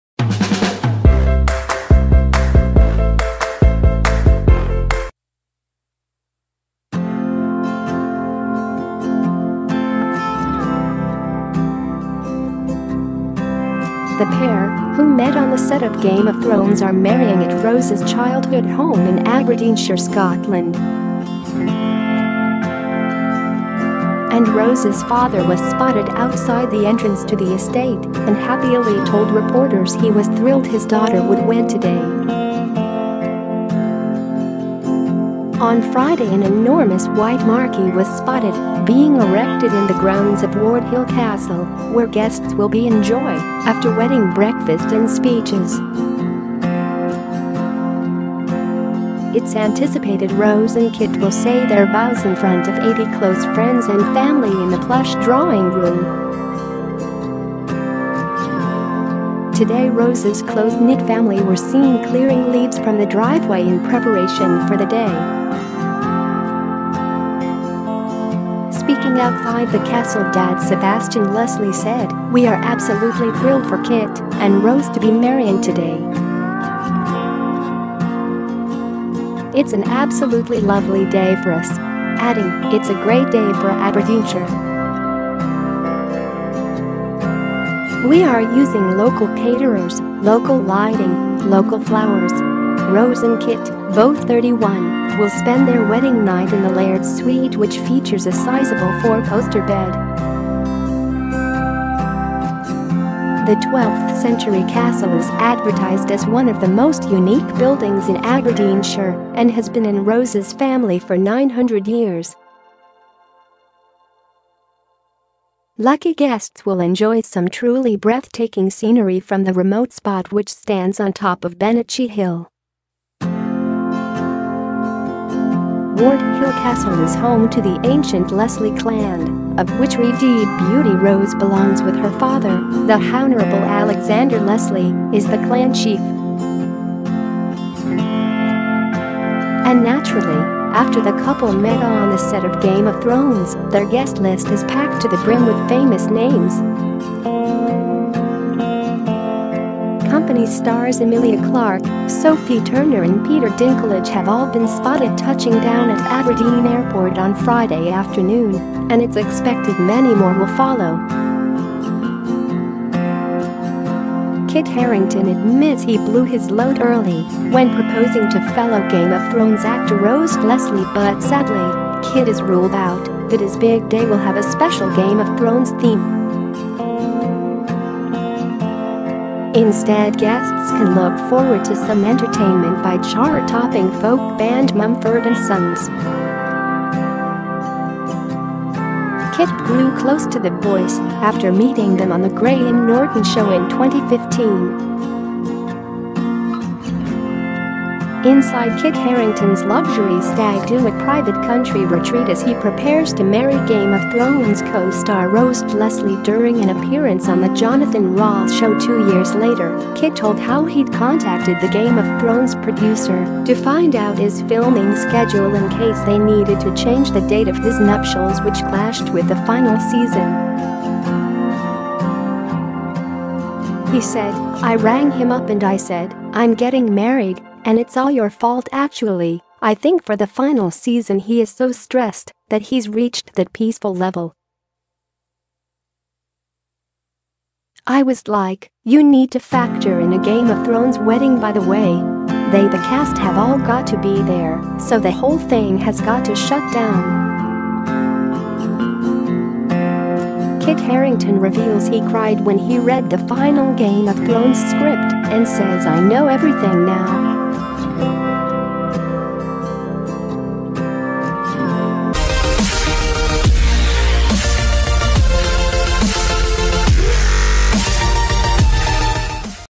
Kit Harington and Rose Leslie wedding - her dad mucks in with final prep - Daily News
Celebrity News,Kit Harington /form Final preparations are underway for Kit Harington and Rose Leslie's wedding - with family members mucking in ensuring the sprawling Scottish castle is in tip top condition.  The pair, who met on the set of Game of Thrones , are marrying at Rose's childhood home in Aberdeenshire, Scotland.  And Rose's father was spotted outside the entrance to the estate and happily told reporters he was "thrilled" his daughter would wed today.  On Friday an enormous white marquee was spotted being erected in the grounds of Wardhill Castle , where guests will be enjoy after wedding breakfast and speeches.   It's anticipated Rose and Kit will say their vows in front of 80 close friends and family in the plush drawing room.  Today Rose's close knit family were seen clearing leaves from the driveway in preparation for the day.  Speaking outside the castle dad Sebastian Leslie said: “We are absolutely thrilled for Kit and Rose to be marrying today. It’s an absolutely lovely day for us.  Adding: “It’s a great day for Aberdeenshire. We are using local caterers, local lighting, local flowers.”   Rose and Kit, both 31, will spend their wedding night in the Laird's suite which features a sizeable four-poster bed.  The 12th century castle is advertised as one of the most unique buildings in Aberdeenshire and has been in Rose's family for 900 years.  Lucky guests will enjoy some truly breathtaking scenery from the remote spot which stands on top of Bennachie Hill.  Wardhill Castle is home to the ancient Leslie Clan, of which redhead beauty Rose belongs - with her father, the Hounourable Alexander Leslie, is the Clan Chief.   And naturally, after the couple met on the set of Game of Thrones, their guest list is packed to the brim with famous names.  Co-stars Emilia Clarke, Sophie Turner and Peter Dinklage have all been spotted touching down at Aberdeen Airport on Friday afternoon and it's expected many more will follow.   Kit Harington admits he 'blew his load early' when proposing to fellow Game Of Thrones actor Rose Leslie    But sadly, Kit has ruled out that his big day will have a special Game of Thrones theme.  Instead guests can look forward to some entertainment by chart-topping folk band Mumford & Sons.  Kit grew close to the boys after meeting them on the Graham Norton show in 2015.   Inside Kit Harington's luxury stag do at private country retreat as he prepares to marry Game Of Thrones co-star Rose Leslie   During an appearance on the Jonathan Ross show two years later, Kit told how he'd contacted the Game of Thrones producer to find out his filming schedule in case they needed to change the date of his nuptials which clashed with the final season.  He said: "I rang him up and I said, 'I'm getting married and it's all your fault actually,'  "I think for the final season he is so stressed that he's reached that peaceful level.  "I was like, 'You need to factor in a Game of Thrones wedding by the way.’ They [the cast] have all got t